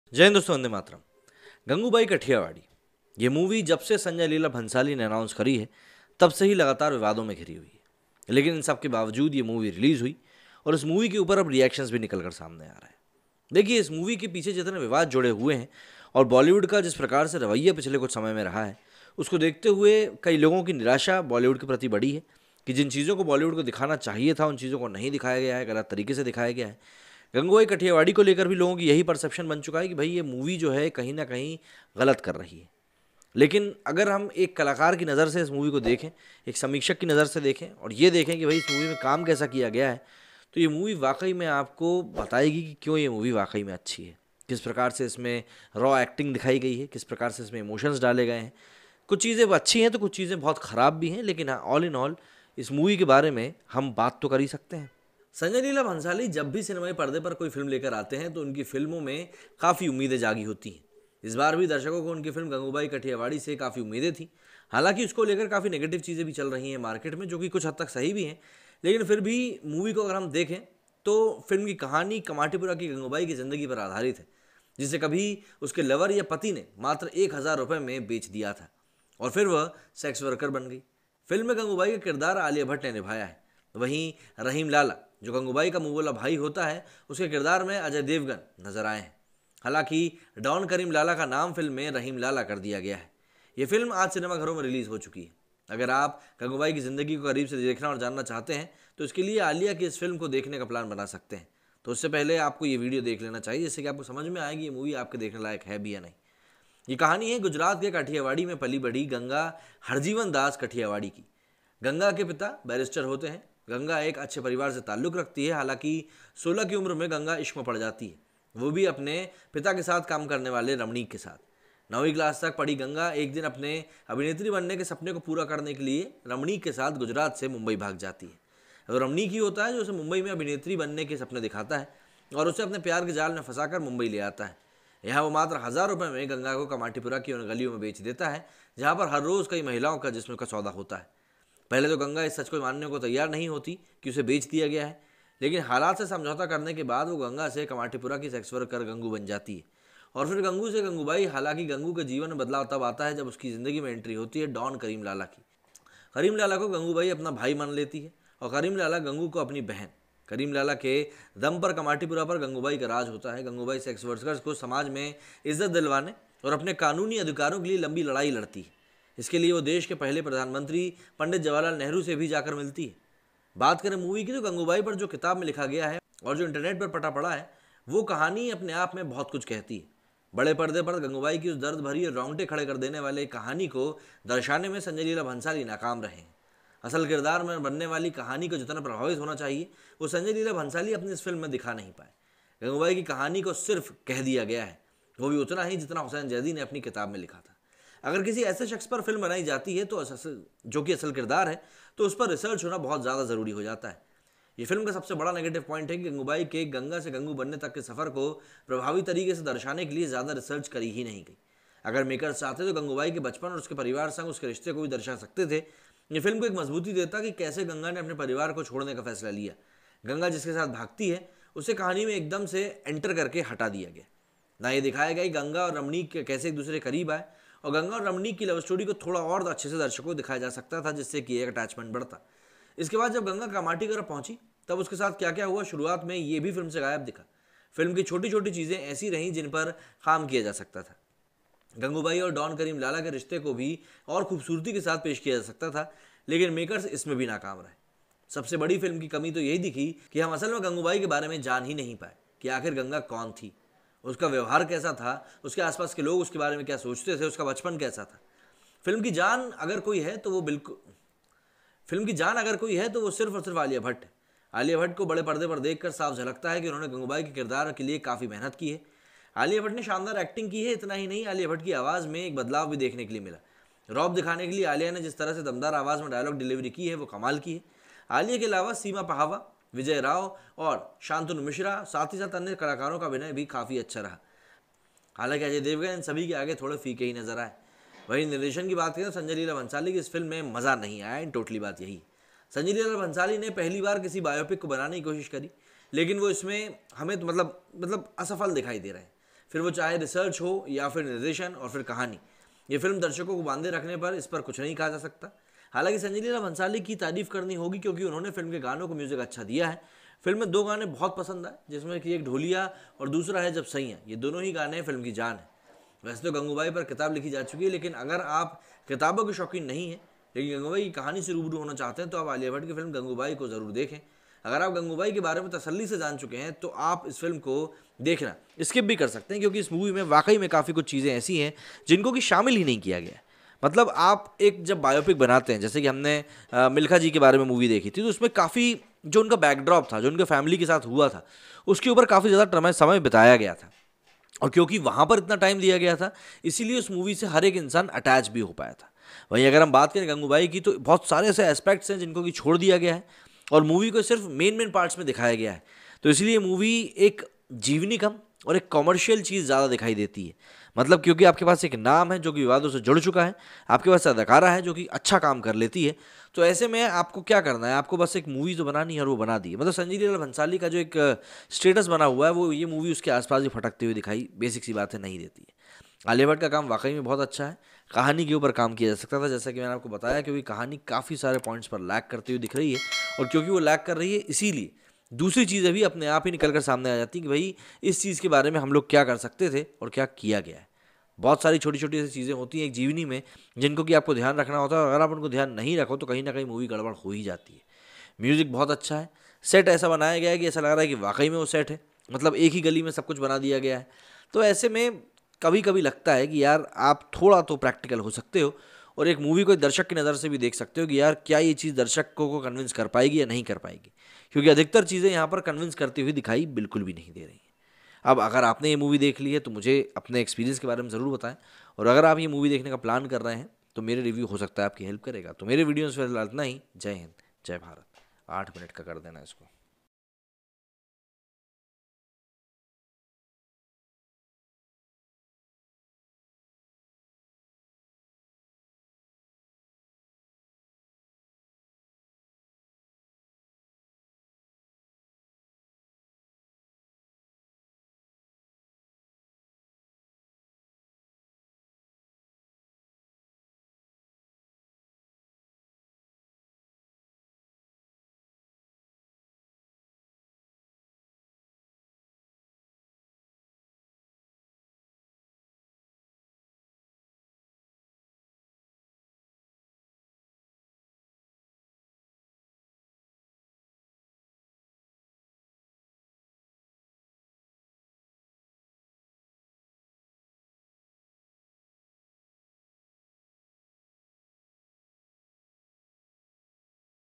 Gangubai Review In Hindi By Naman Sharma । Review Point
Gangubai Kathiawadi movie review: Sanjay Leela Bhansali’s latest is the kind of old-fashioned dialogue-heavy, sentiment-on-sleeve film which Bollywood is forgetting how to make.